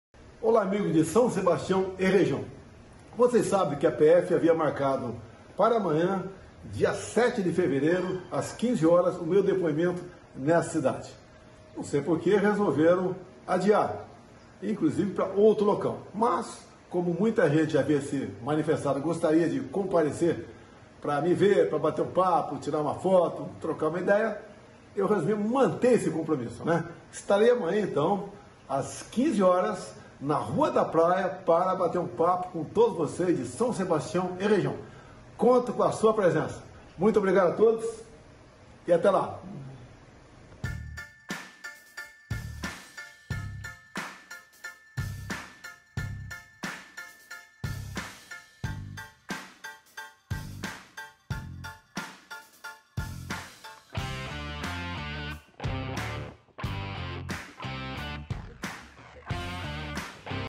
COM BALEIA DE FUNDO, BOLSONARO MARCA ENCONTRO COM APOIADORES ONDE SERIA OUVIDO PELA PF
A Polícia Federal (PF) adiou o depoimento do ex-presidente Jair Bolsonaro (PL) sobre o inquérito que apura se ele importunou uma baleia jubarte durante um passeio de jet-ski em São Sebastião, no litoral norte de São Paulo.